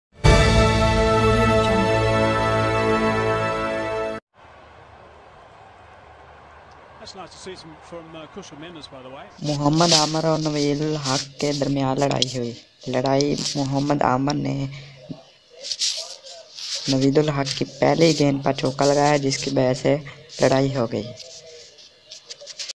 Naveen Up Haq Vs Muhammad Amir Fight In LPL T20 2020
Naveen Ul Haq Vs Muhammad Amir Fight In LPL T20 2020